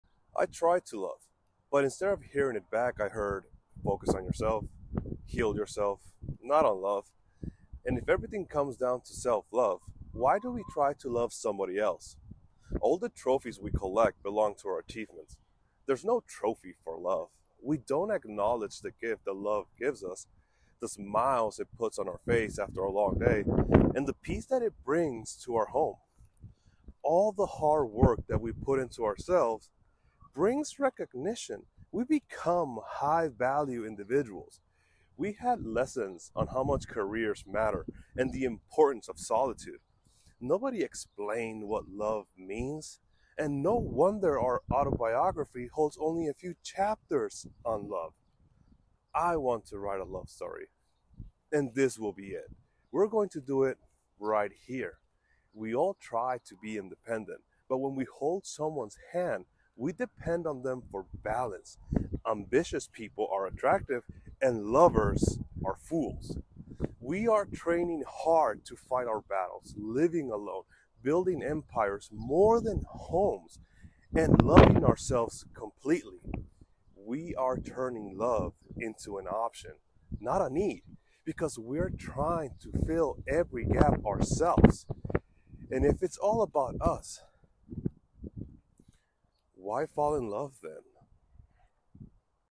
Love is not an option.
Love seems elusive sometimes. We try to give it, and all too often receive only empty advice in return. But at the end of the day, it’s important to remember that love is its own reward. It may not come with a trophy, but it’s worth cherishing and appreciating all the same.